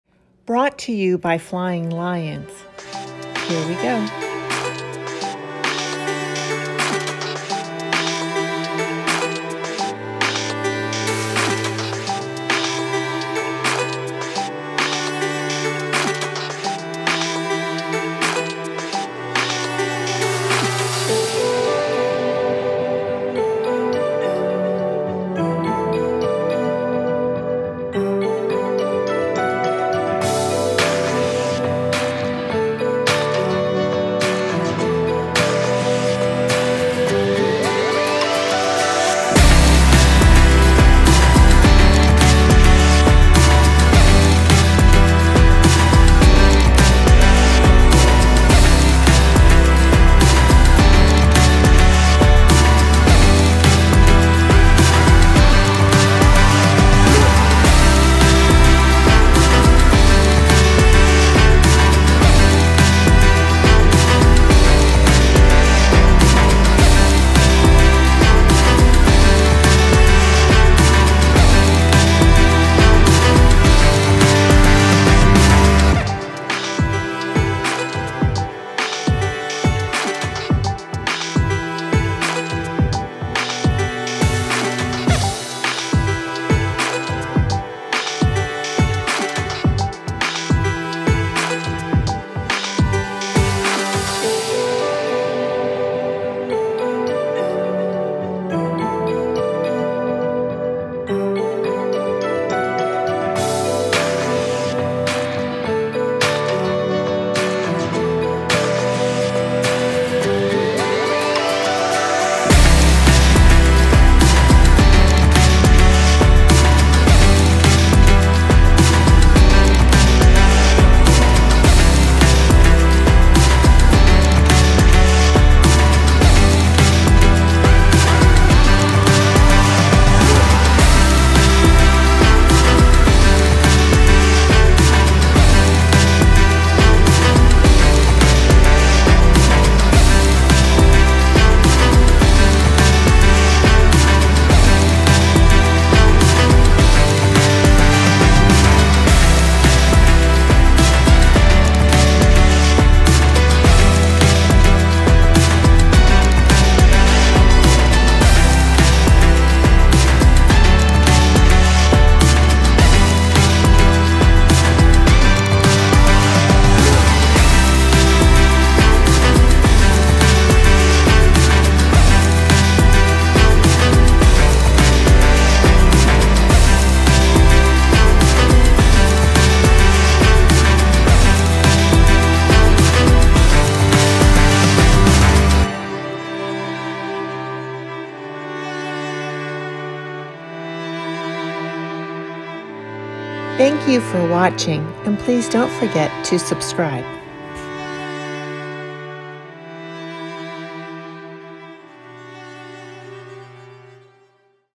DRONE VIEW 🌀DAMAGES in Vero Beach after Hurricane NICOLE 🌨️🌊
We enjoy capturing moments with our life.  Thank you for taking time to watch our videos. 🙏

Crazy flooding & damage from this storm 🌀 in our hometown Vero Beach, Florida.  Video of Huminston, Jaycee, Causeway, Riverside.  Video Taken Beachside by Kevin Lyons.  Editing by Lanette Lyons.